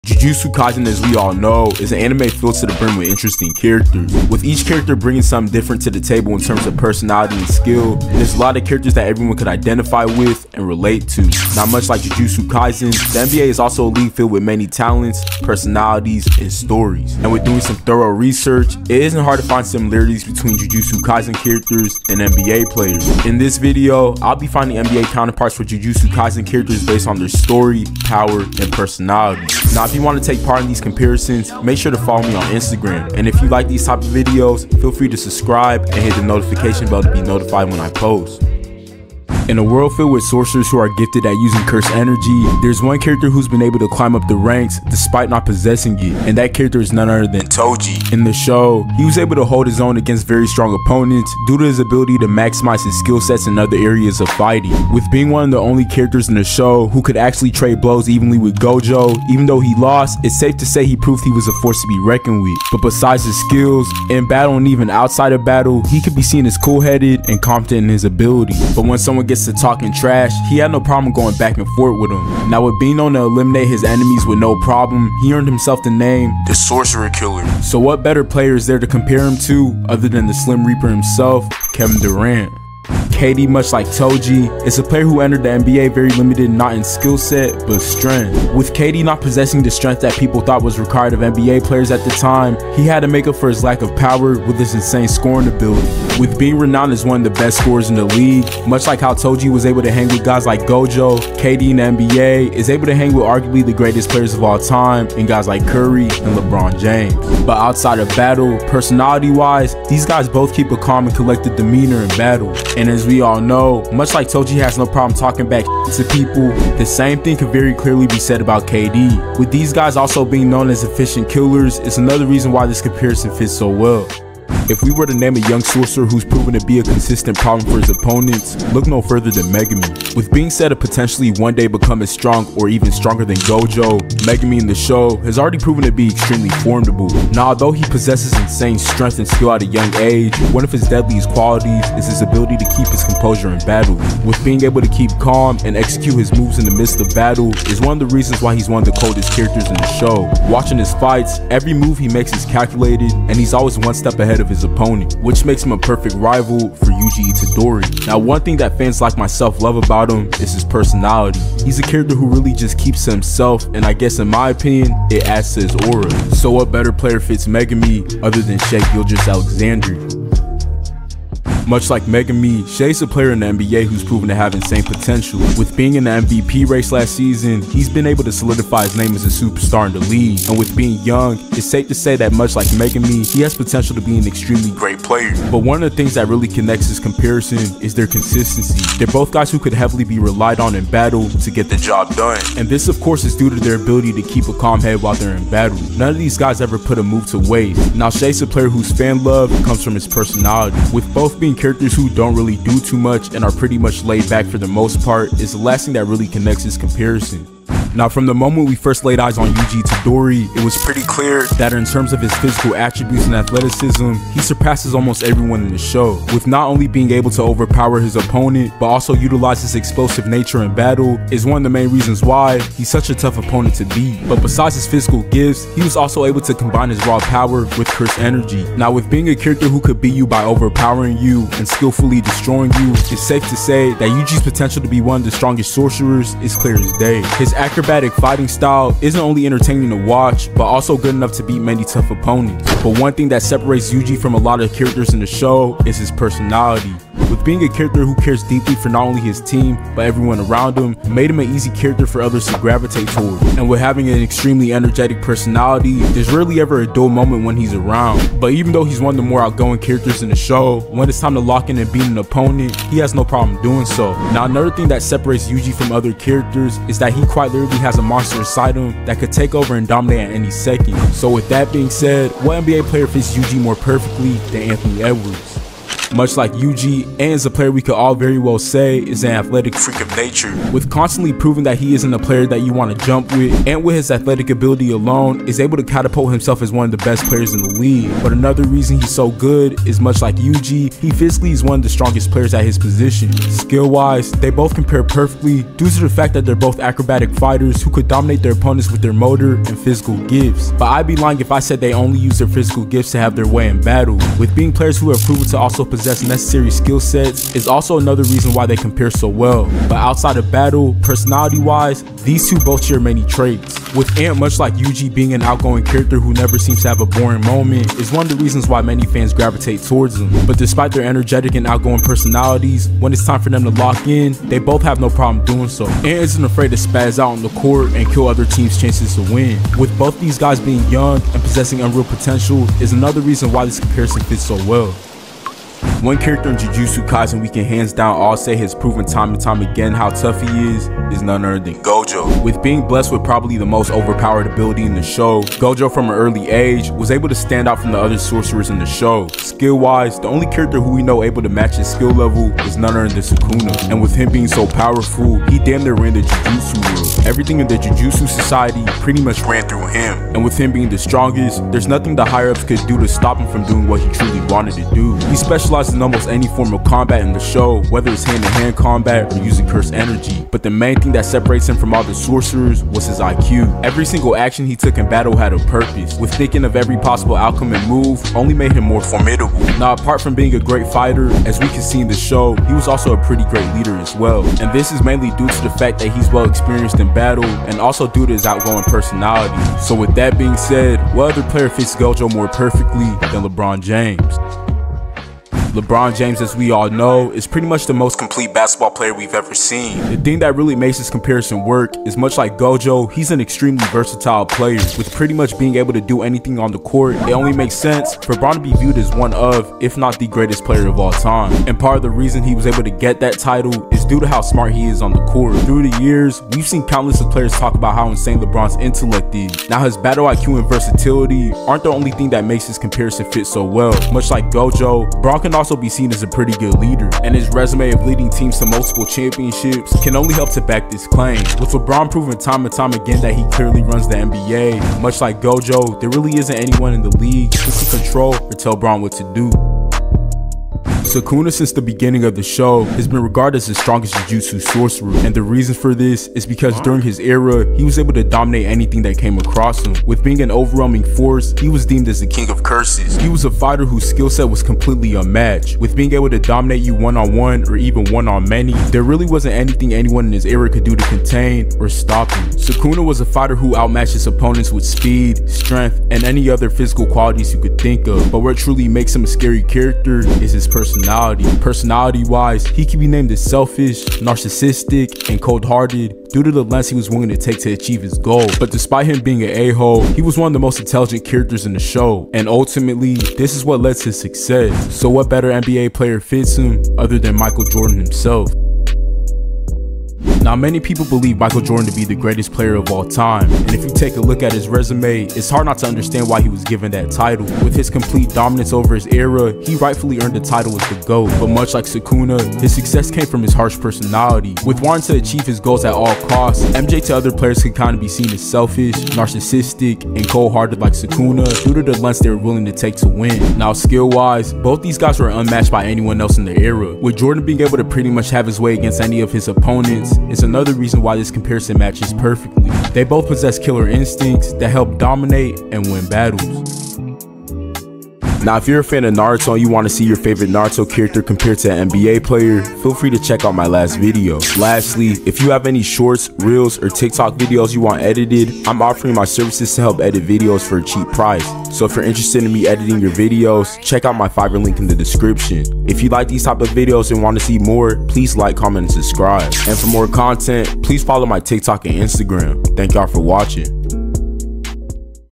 What if NBA Players were in Jujutsu Kaisen?!
Jujutsu Kaisen as we all know is an anime filled to the brim with interesting characters. With each character bringing something different to the table in terms of personality and skill. There’s a lot of characters that everyone can identify with and relate to. Now much like JJK, the the NBA is also a league filled with many talents, personalities and stories. And with doing some thorough research It isn’t hard to find similarities between jujutsu Kaisen characters and NBA players. In this video I’ll be finding NBA counterparts for JJK characters based on their story, power, and personality.

(Matchupsmedia)